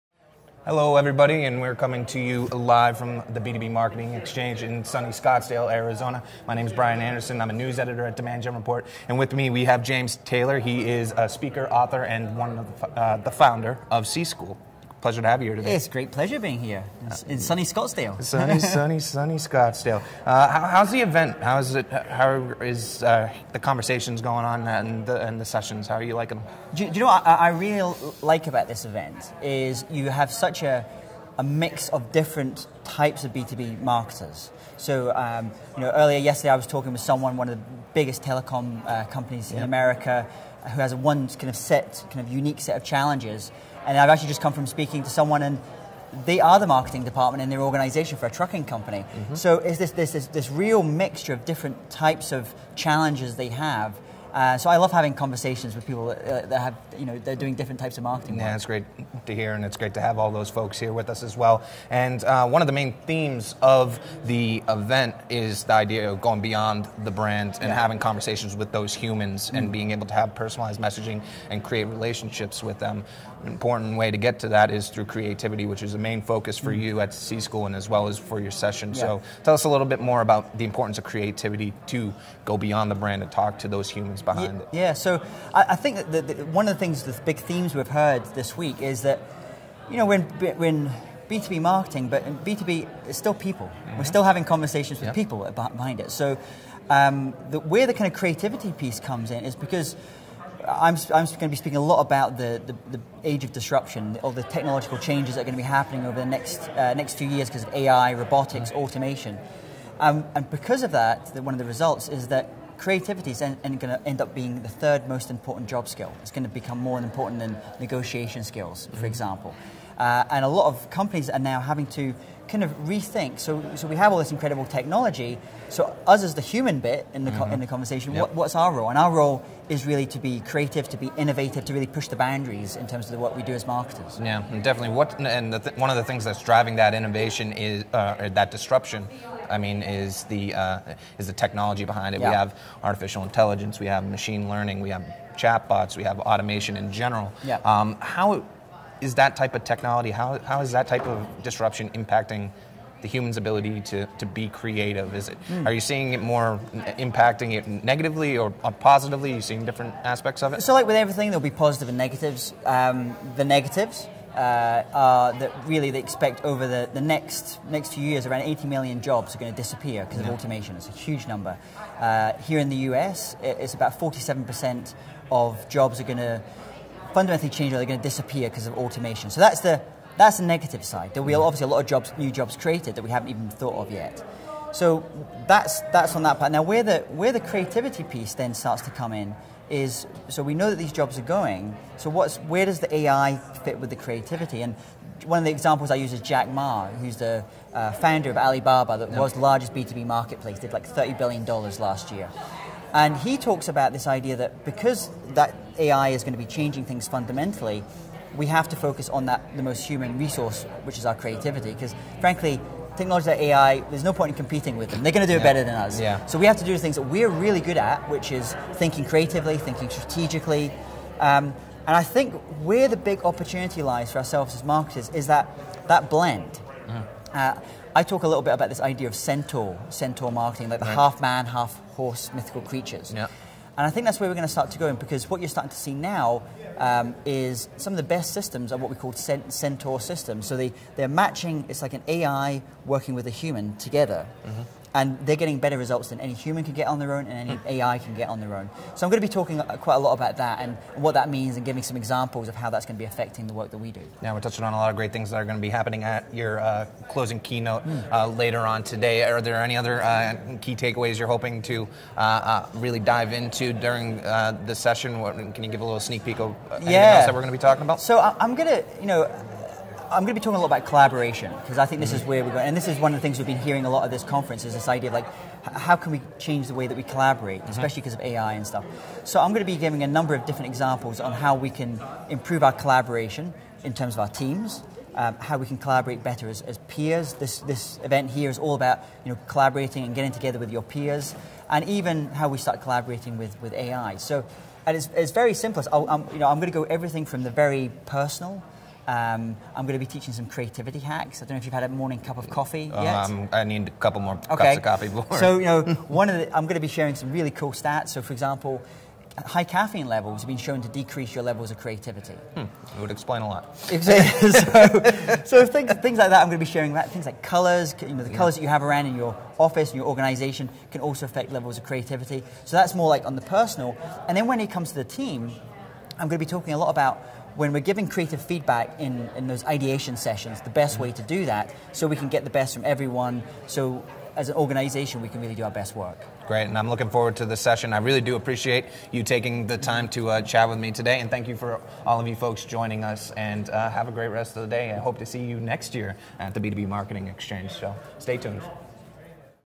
The Effect Of Technology On Human Creativity
Due to the age of technological disruption, creativity will soon be a top priority job skill as B2B marketers navigate the symbiosis between artificial intelligence and classic marketing techniques.

At the B2B Marketing Exhange, Demand Gen Report's Brian Anderson and keynote speaker James Taylor discussed how technology effects human creativity and how we can improve collaboration techniques not only between people, but between humans and bots.

Check out the podcast for more insight from Taylor.